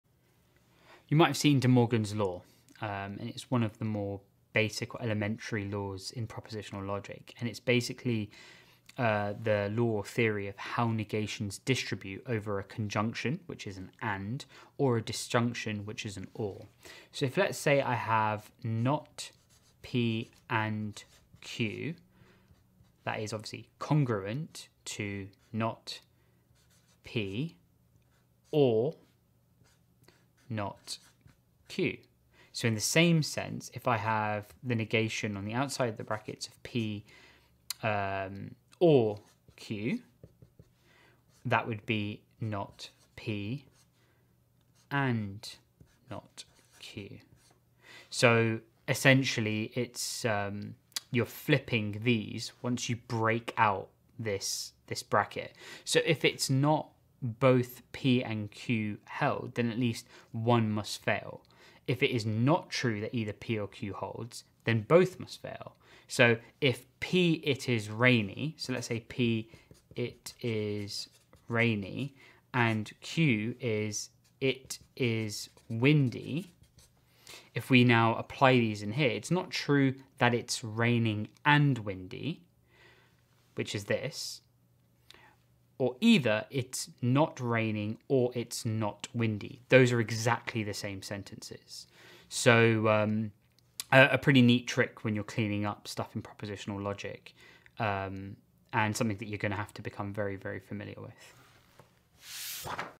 Propositional Logic: De Morgan's Law
Breaking down De Morgan's Law in under 2 minutes like an absolute beast.